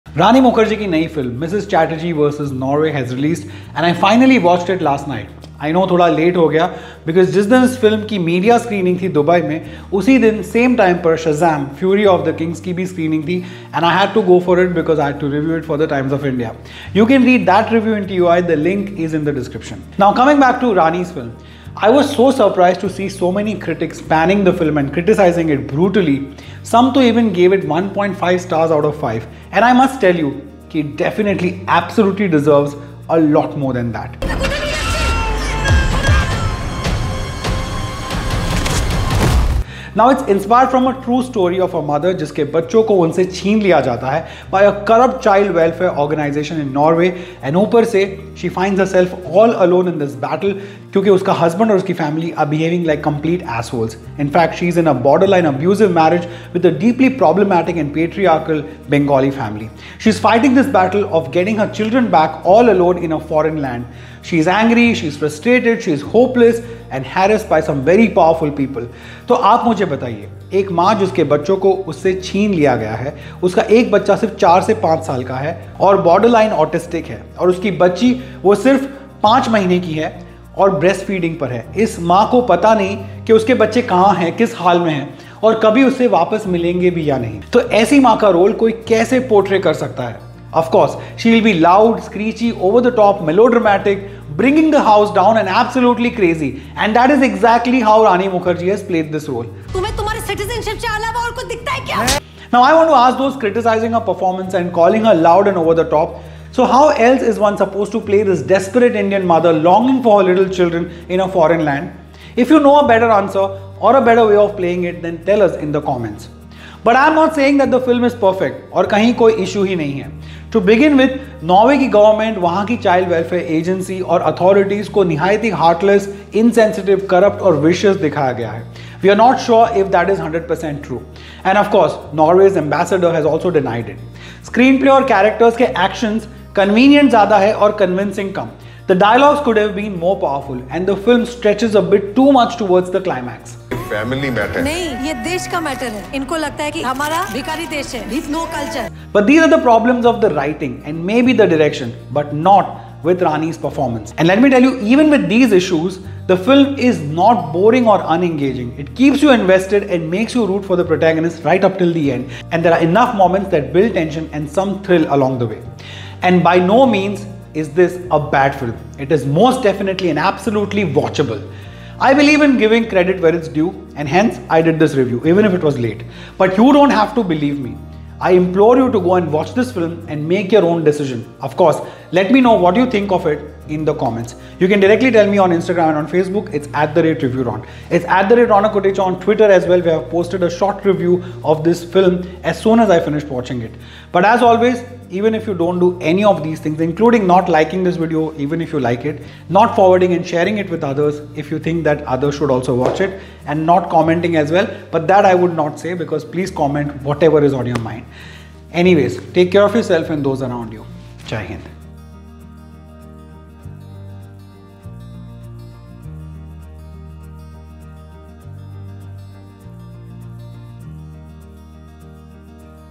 Why This Film is a Must Watch | Mrs Chatterjee vs Norway Real Review | Ronak Kotecha
Mrs Chatterjee vs Norway, is this movie really that bad? 1.5 stars - is it really the kind of rating this movie deserves? Checkout the full video to know more.

Support our Backup Channel @ronversations
Video Editing & Graphics Credits:
Video is edited by the team of  @harsh_khatuja